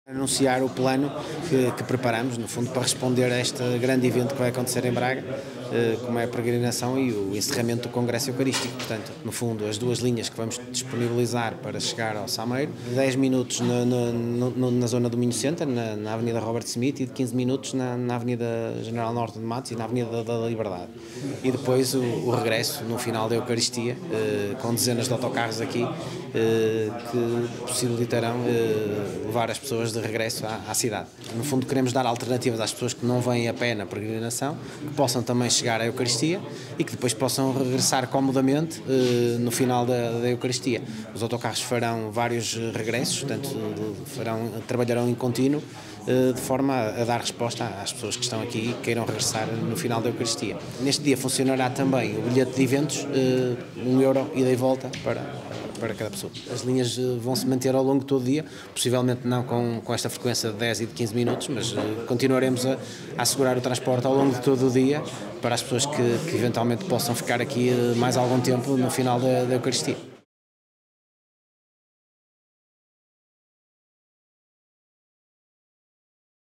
O administrador dos TUB, Teotónio dos Santos, fala da operação para o encerramento do 5º CEN
O administrador dos Transportes Urbanos de Braga dia que haverá uma operação específica para transportar fiéis que não seguirão a procissão para o Santuário do Sameiro, a partir das 7h do dia 2 de junho, para o encerramento do 5º Congresso Eucarístico Nacional.